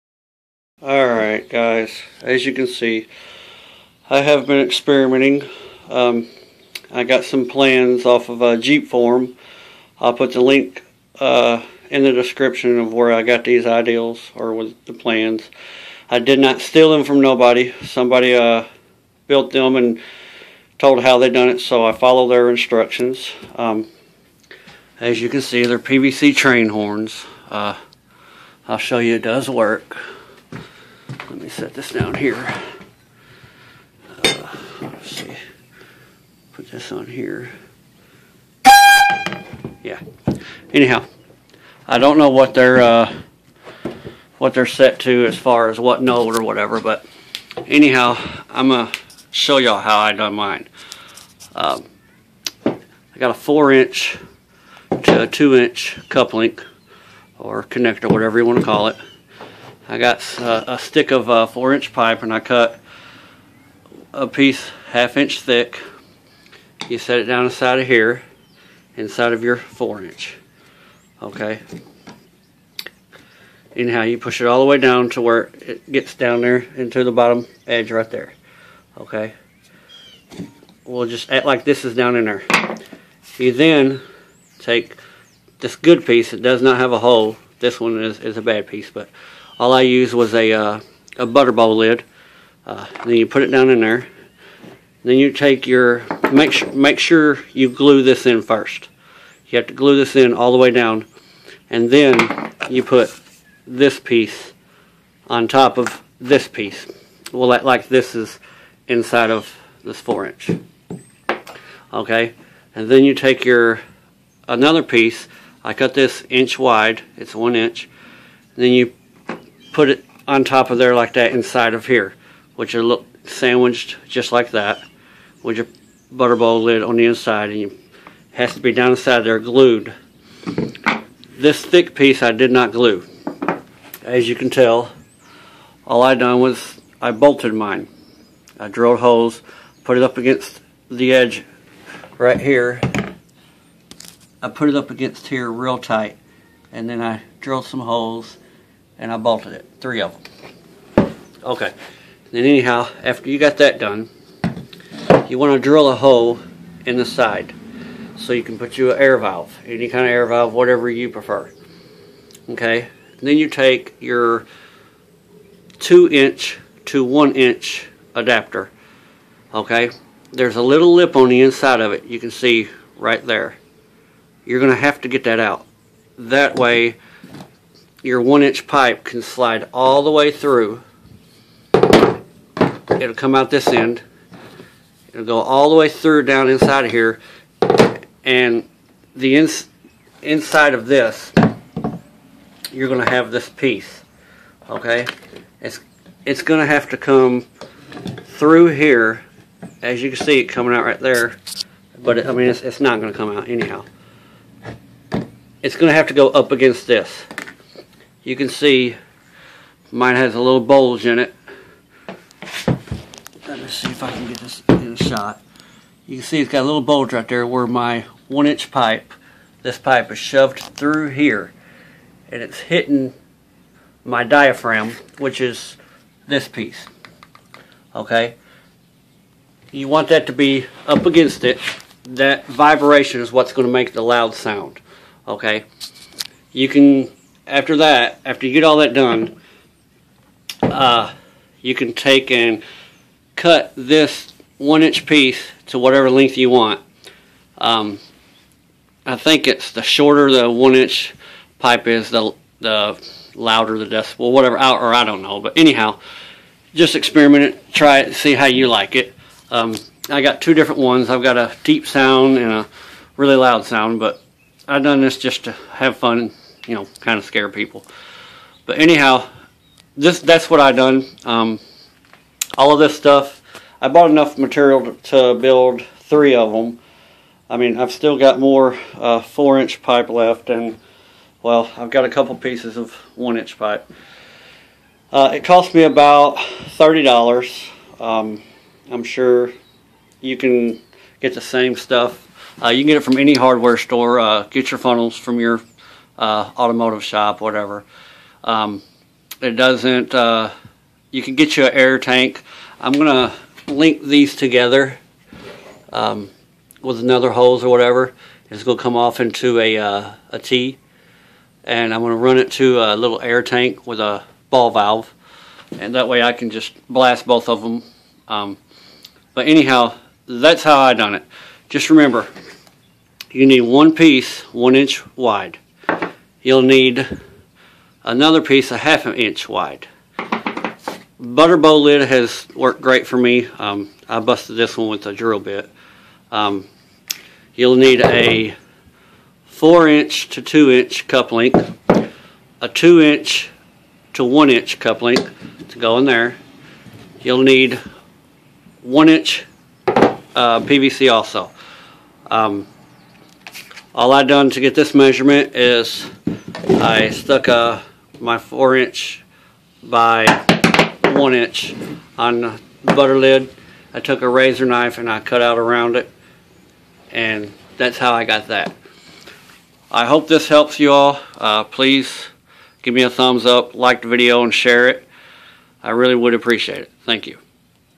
How To Make A PVC Train Horn (For Real)
The Website That I Got This From Is I Did Not Steal The Plans As You Can See If You Go To The Site It Will Tell You How To Make It. I Just Made A Video Showing You How.

ATTENTION

BUILD AT YOUR OWN RISK!
I TAKE NO FAULT OR RESPONSABILITY
FOR ANY DAMAGES TO YOU OR YOUR PROPERTY!
IF YOU CHOSE TO USE SOME OF MY IDEA'S
IT IS AT YOUR OWN RISK...
PLEASE BE SAFE, AND REMEMBER MODIFYING ANYTHING
COMES WITH GREAT RISK!!!

Items You Will Need Are (For One Train Horn)
1 4in To 2in Cup-link
1 2in To 1in Adapter
1 Stick Of 4in PVC Pipe
1 Stick Of 1in PVC Pipe
1 Transmission Funnel
1 Butter Bowl Lid
1 Air Valve
And Some PVC Glue

Instructions are
1st Cut 1 1/2in and 1 1in Piece Of 4in PVC.

2nd Take Your Butter Bowl Lid And Put Your 1in By 4in On It And Cut Around It.

3rd Drill A Hole Big Enough For Your Air Valve In The 4in To 2in Cup-link And Put It In.

4th Glue Your 1/2in By 4in Into The 4in To 2in Cup-link Making Sure You Have It Pushed As Far Down Inside Of It As It Can Go.

5th Put Your Cut Out Butter Bowl Lid On Top Of The 1/2in By 4in That You Just Glued Inside Of The 4in To 2in Cup-link.

6th Put Your 1in by 4in Piece On Top Of The Cut Out Butter Bowl Lid Making Sure Its Tight Against The Lid.

7th Mark And Drill 3 Hols To Hold The 1in By 4in Tight Against The Butter Bowl Lid And Put Your Bolts In And Tighten Them.

8th Carve Out The Lip Inside Of The 2in To 1in Adapter So That The 1in PVC Pipe Will Slide All The Way Through Without Stopping And Then Glue The 2in To 1in In The 4in To 2in Cup-link.

9th Put Your 1in PVC Pipe Into The 2in To 1in Adapter And Push In Untell It Touches The Butter Bowl Lid But It Needs To Be Against It With A Little Pressure And Glue The 1in PVC In Place.

10th Cut The Funnel To Slid Tightly On The 1in PVC Pipe.

11th Let All Dry And Have Some Fun.

Build Time Took Me About 1hr To Cut And Make Everything.

Many Many Thanks To The Man On The Jeep Forums That I Found. Your Instructions Were Great And Easy.

I Hope This Helps Everyone Out Please Rate Comment And Sub. P.S. Sorry For The Bad Spelling And Rough Directions